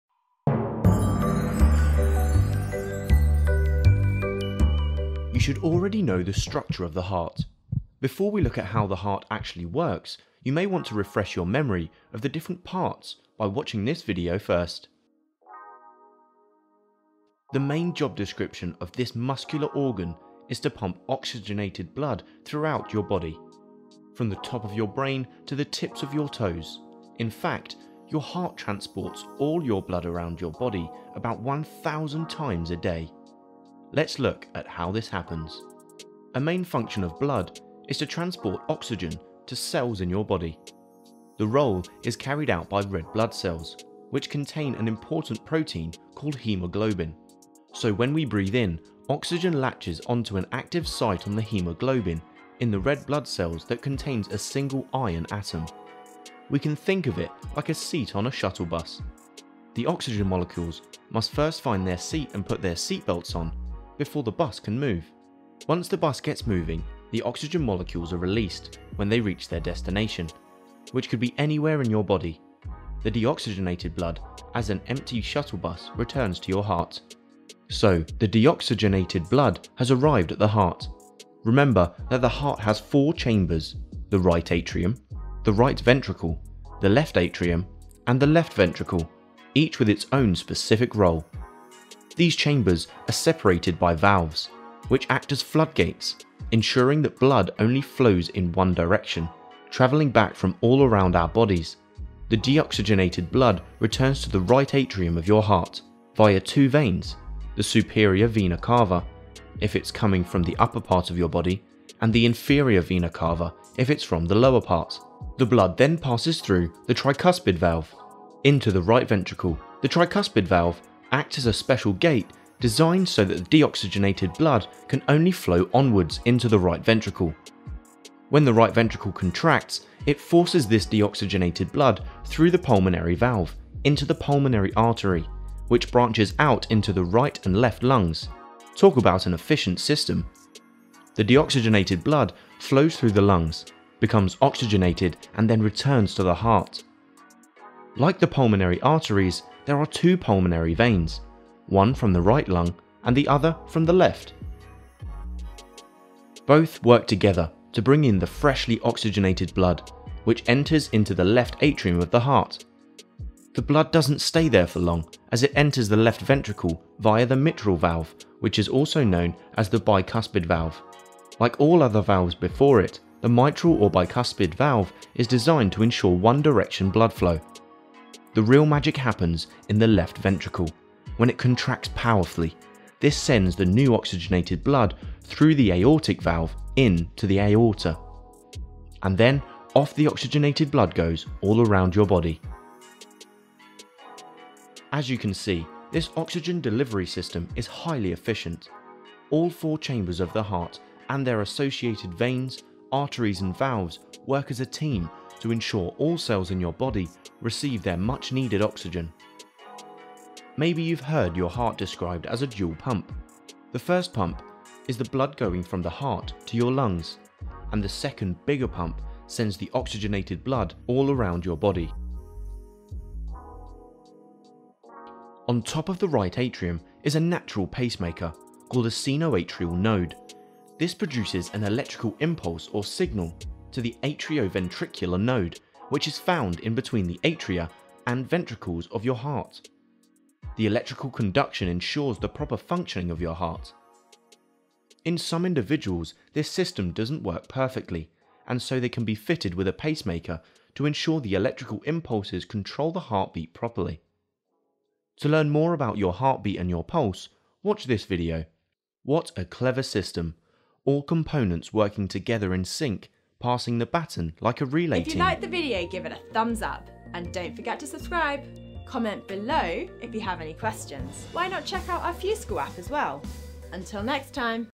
How the Heart Works | Physiology | Biology | FuseSchool
The main job description for this muscular organ is to pump oxygenated blood throughout your body... from the top of your brain to the tips of your toes… in fact, your heart transports all your blood around your body about 1000 times a day!! In this video we are going to explore how the heart works.

CREDITS
Animation & Design: Bing Rijper
Narration: Dale Bennett
Script: Amanda Edward